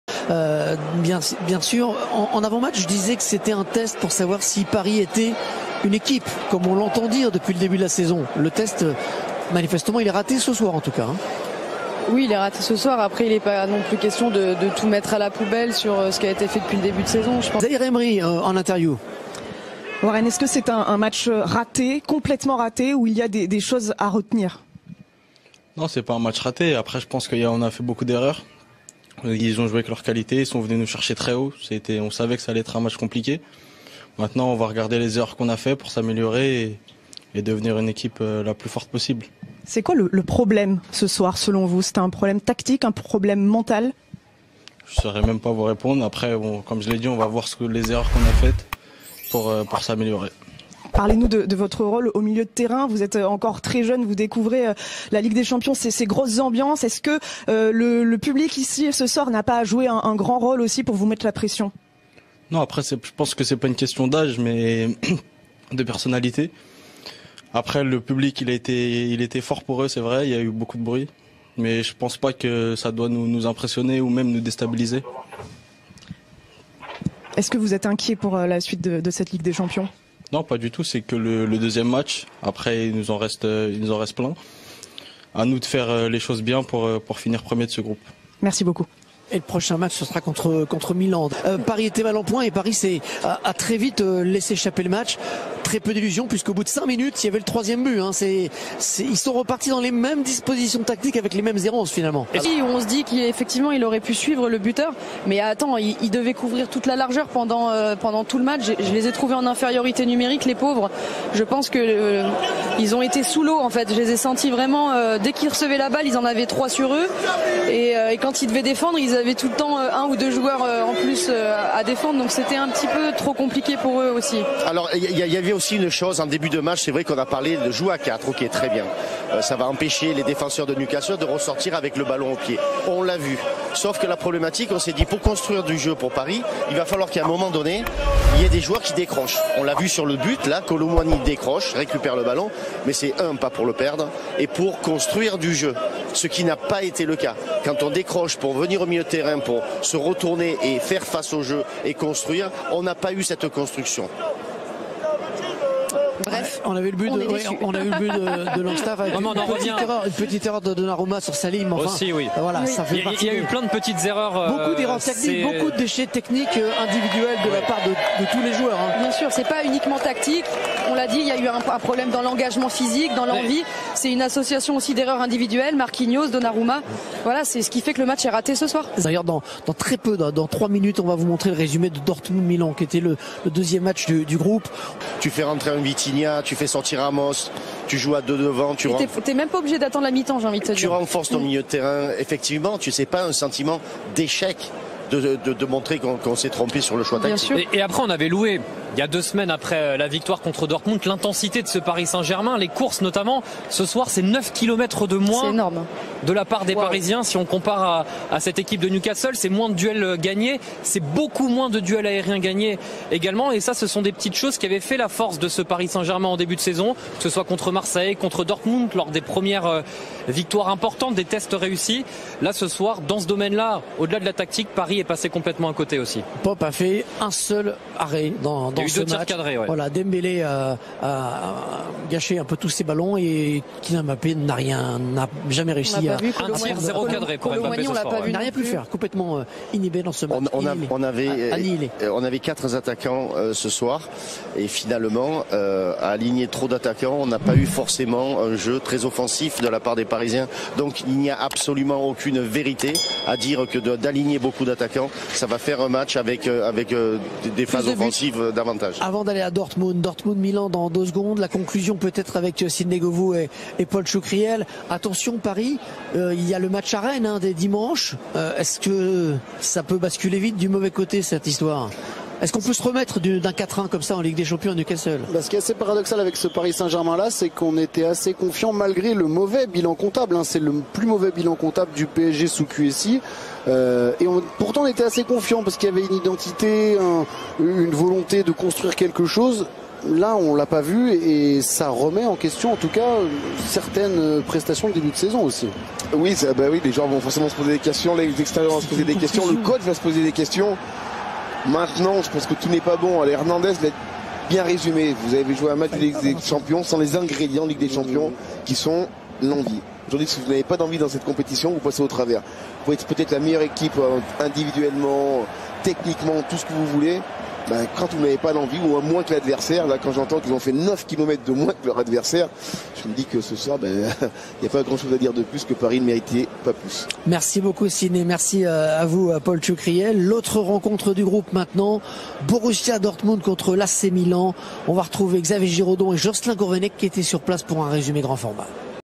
Laure Boulleau : Le PSG face à Newcastle était très fragile, Le Club besoin d'un Coach Français !
Le Paris Saint-Germain a complètement pris l'eau à Newcastle lors de la 2e journée de la Ligue des champions 2023/2024, battu 4-1 à St. James' Park.
D'abord plombé par une erreur de Marquinhos qui a conduit au but d'Almiron, Paris a également cédé avant la mi-temps sur une réalisation discutable de Burn. Puis Longstaff a marqué le 3e but avant la réduction du score de Lucas Hernandez...pour rien. C'est même le défenseur Schär qui a permis aux Magpies de finir en beauté
Mbappé, inexistant, ne s'est pas procuré une occasion, tandis qu'Ousmane Dembélé a été particulièrement maladroit dans ses tentatives.